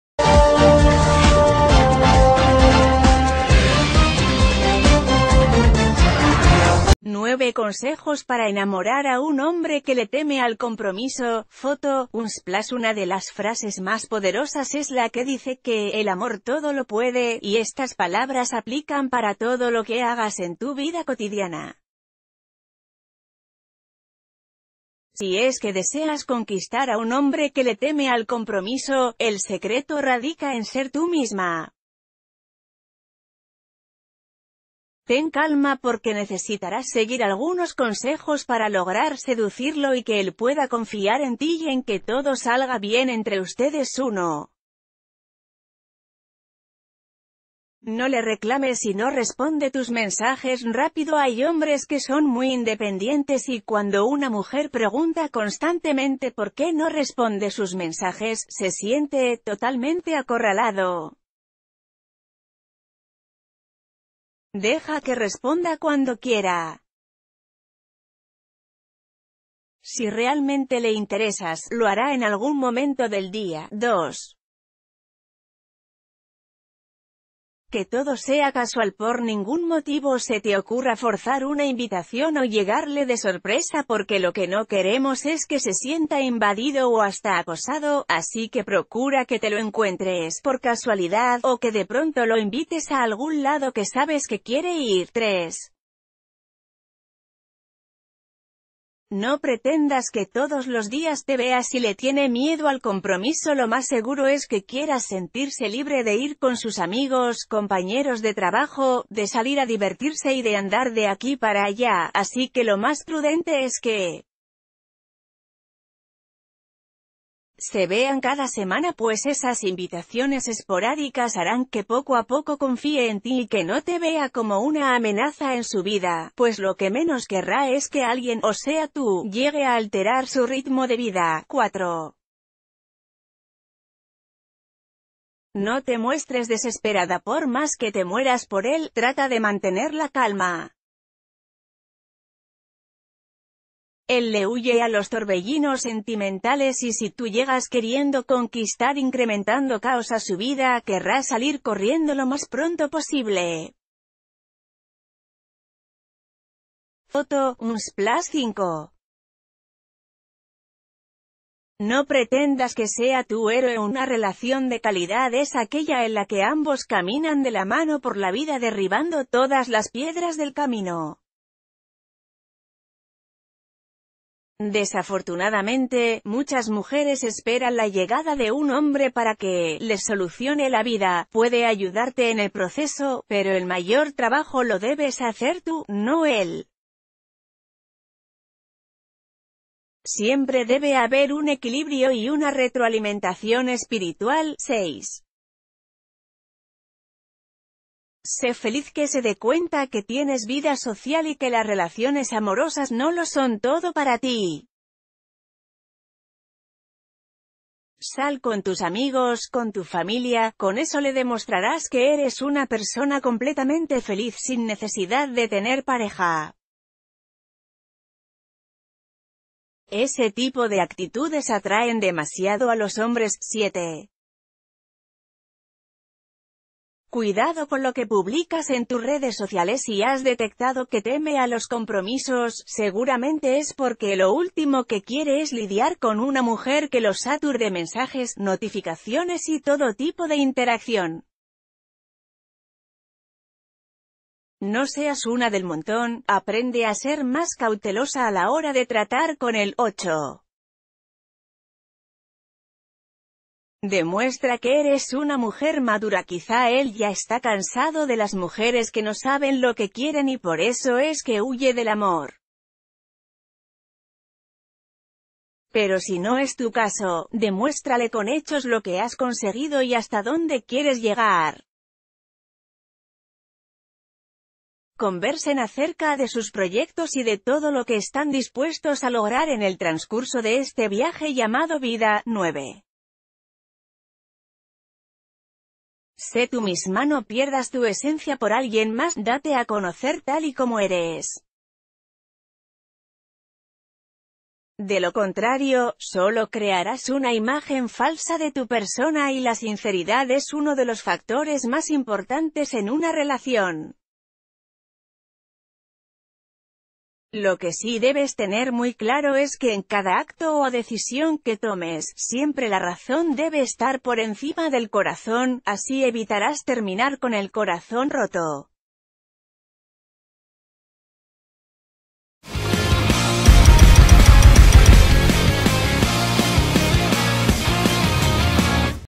9 consejos para enamorar a un hombre que le teme al compromiso
9 consejos para enamorar a un hombre que le teme al compromiso

9 consejos para enamorar a un hombre que le teme al compromiso | Foto: Unsplash Una de las frases más poderosas es la que dice que "el amor todo lo puede" y estas palabras aplican para todo lo que hagas en tu vida cotidiana. Si es que deseas conquistar a un hombre que le teme al compromiso, el secre...